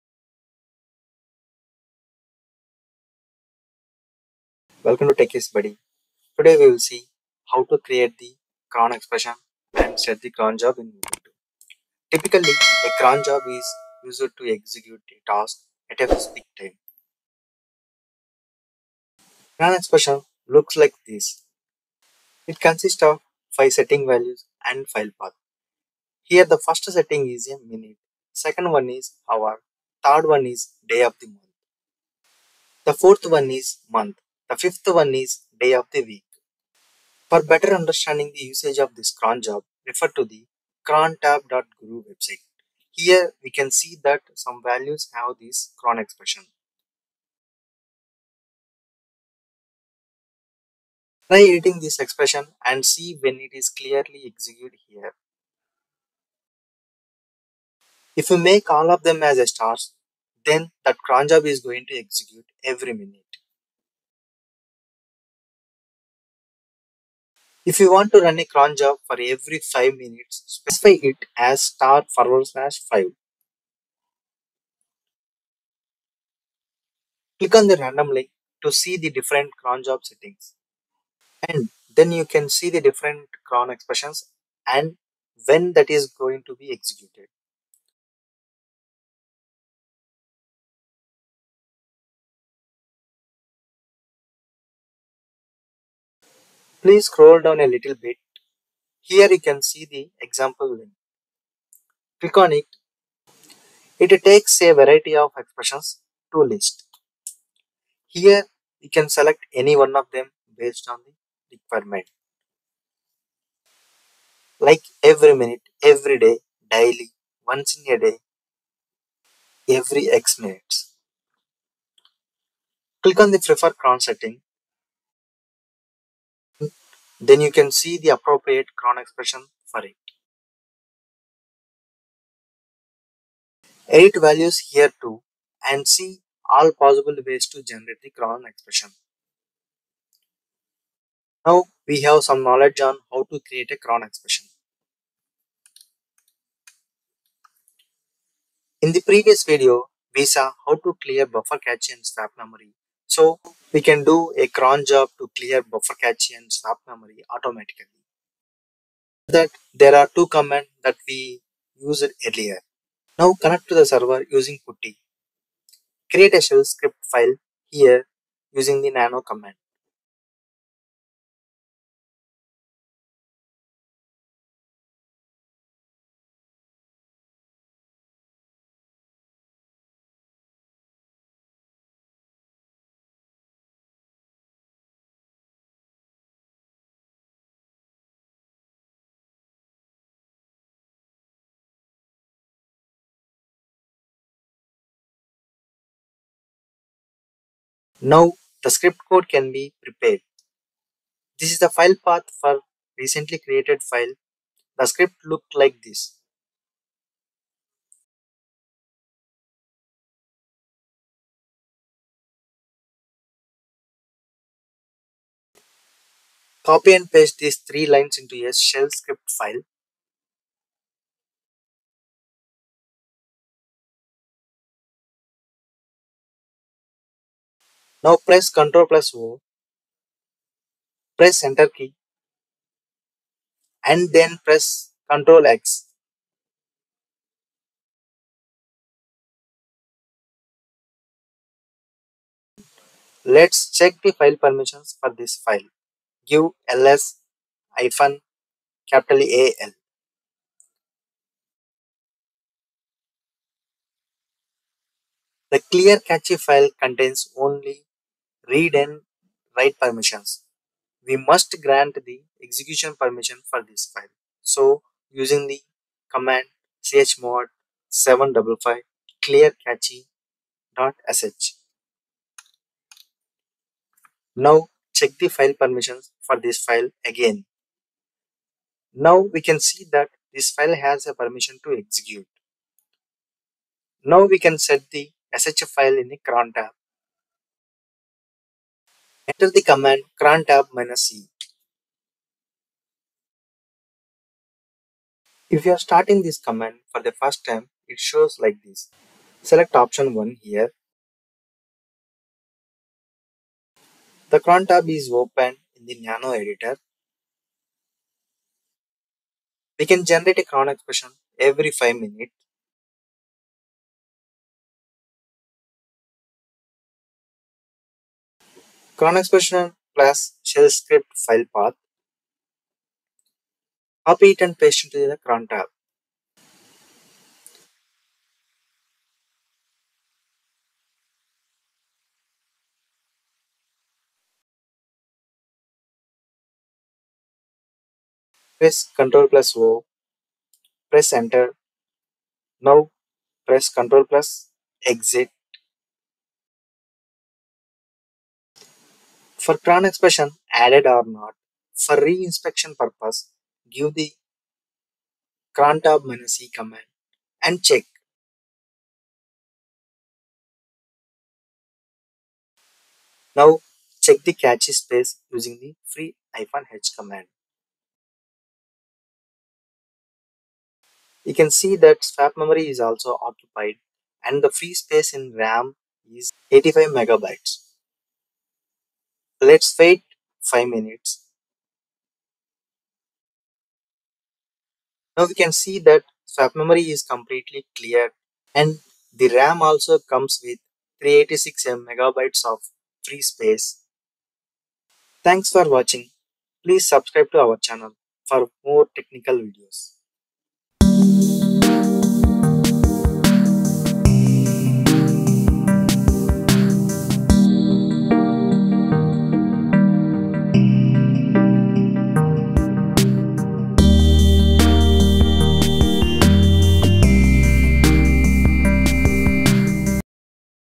How To Create The Cron Expression & Set The Cron Job In Ubuntu/Linux
Hi Friends,

For Clearing pagecache, dentries, and inodes.

echo 3 {use gaterthan symbol}/proc/sys/vm/drop_caches

For Clearing Swap Space

swapoff -a && swapon -a